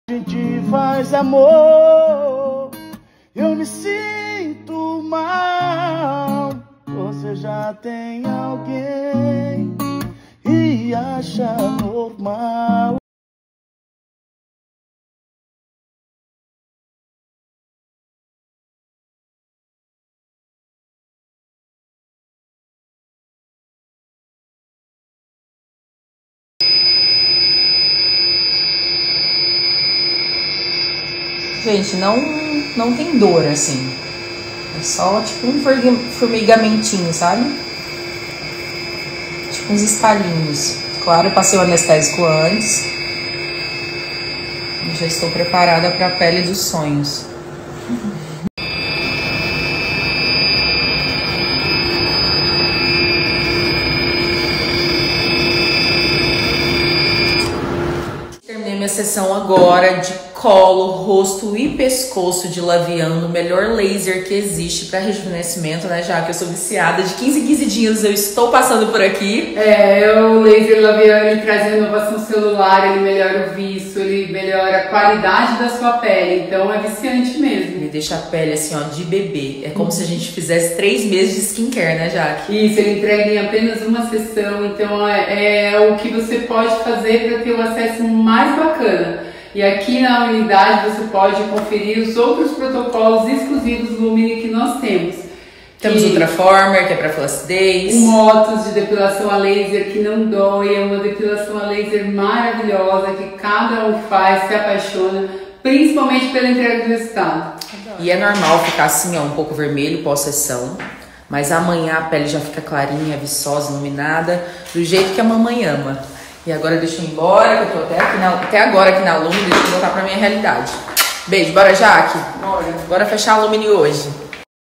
Gusttavo Lima APAIXONADO cantando ao violão e Andressa Suita diz “do jeito que a mãe gosta”
Gusttavo Lima cantando música apaixonada enquanto faz companhia para os filhos e Andressa Suita surge em Goiânia e fala com os fãs durante o dia de princesa kkk